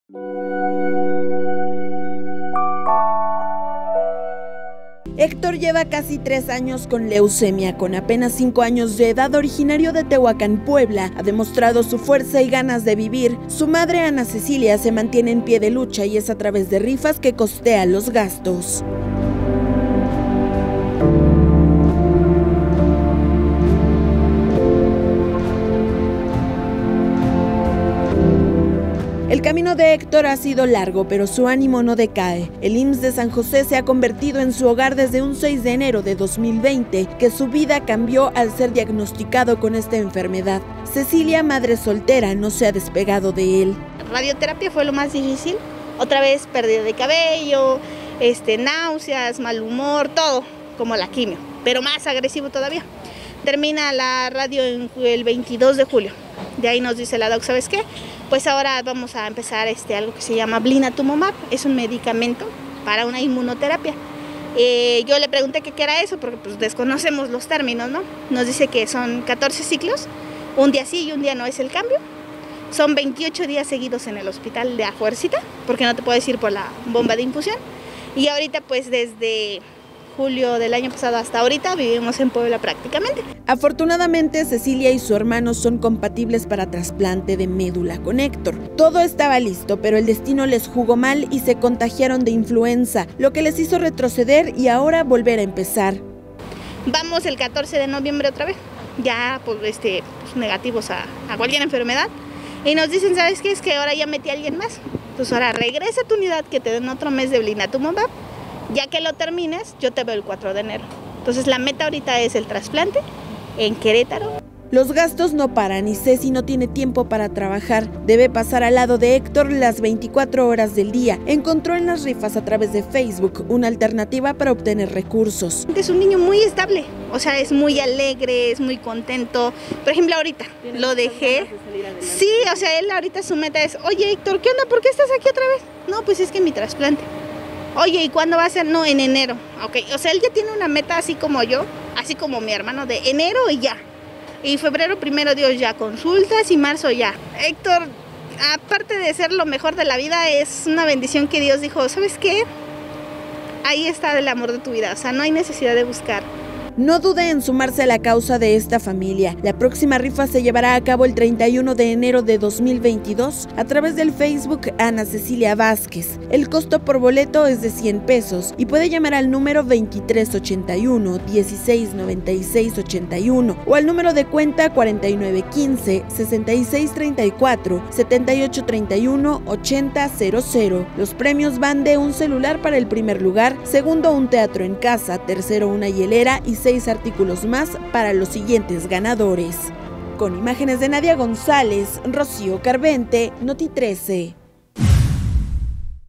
🔴 La mamá del pequeño Héctor necesita ayuda para el tratamiento de cáncer que padece.
PueblaSolidaria 🔴 “Radioterapia fue lo más difícil, pérdida de cabello, náuseas, malhumor, todo, pero más agresivo”, Así han sido tres de los cinco años del pequeño Héctor, por el cáncer que padece; su madre, soltera, solo vive para él y se apoya con rifas para solventar sus gastos. Si puedes apoyarlos, marca al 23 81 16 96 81 o deposita a Banorte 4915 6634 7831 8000.